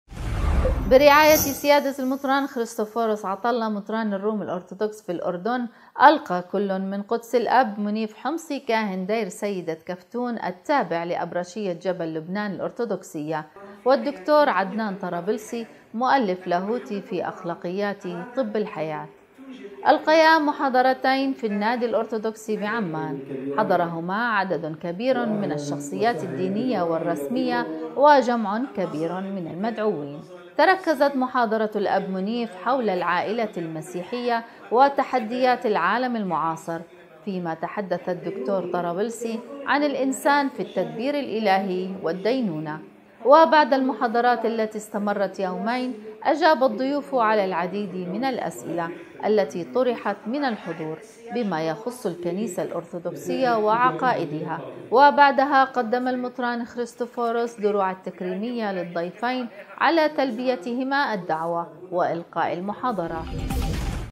نورسات الأردن : برعايةِ سيادةِ المطران خريستفورس القيت محاضرتينِ في النادي الارثوذكسي بعمان
برعايةِ سيادةِ المطران خريستفورس مطرانِ الرومِ الارثوذكس في الاردن / ألقى كلٌ من قدسِ الأب منيف حمصي كاهنِ دير سيدةِ كفنون / التابعِ لأبرشيةِ جبلِ لبنان الارثوذكسيه / والدكتور عدنان طوباسي مؤلفْ لاهوتي في أخلاقياتِ طبِ الحياه / ألقيا محاضرتينِ في النادي الارثوذكسي بعمان / حضرهما عددٌ كبيرٌ من الشخصياتِ الدينيةِ والرسميه / وجمعٌ كبيرٌ من المدعوينَ وأبناءِ الرعيه .. تركزتْ محاضرةُ الأب منيف / حولَ العائلةِ المسيحيه وتحدياتِ العالَمِ المعاصر / فيما تحدث الدكتور طوباسي عن الانسانِ في التدبيرِ الإلهي والدينونه / وبعد المحاضراتِ التي استمرت يومين / أجاب الضيوفُ على العديدِ من الأسئلةِ التي طُرحت من الحضور / بما يخصُ الكنيسةَ الأرثوذكسيةَ وعقائدَها / وبعدها قدمَ المطران خريسفورس الشكرَ للضيفين / على تلبيتهما الدعوةَ والقاءِ المحاضرات